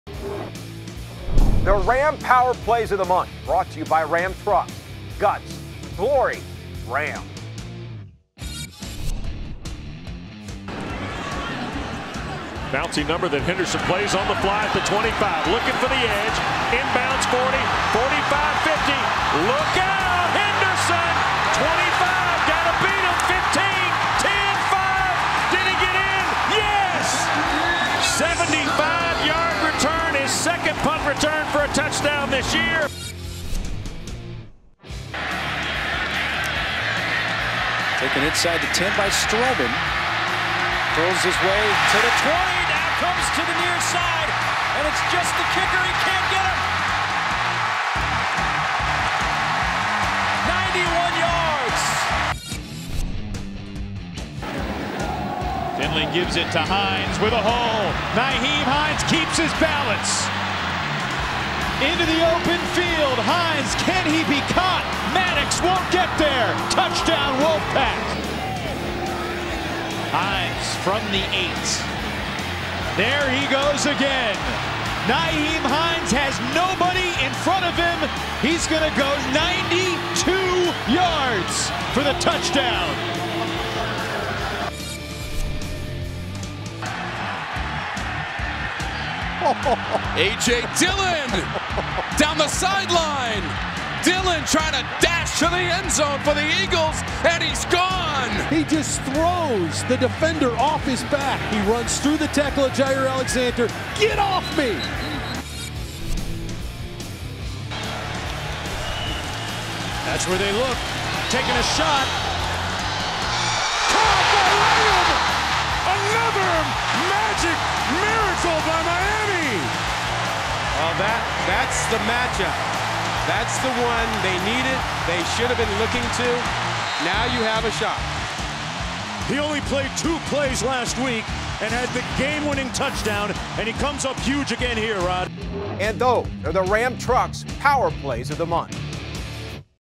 Best Plays of October 2017 | RAM Trucks Power Plays of the Month
Here are the best plays from ACC football in the month of October, featuring a miraculous moment in Miami, NC State's Nyheim Hines breaking free in the open field and a stiff arm from AJ Dillon that is unlike any other.  These are the RAM Trucks Power Plays of the Month.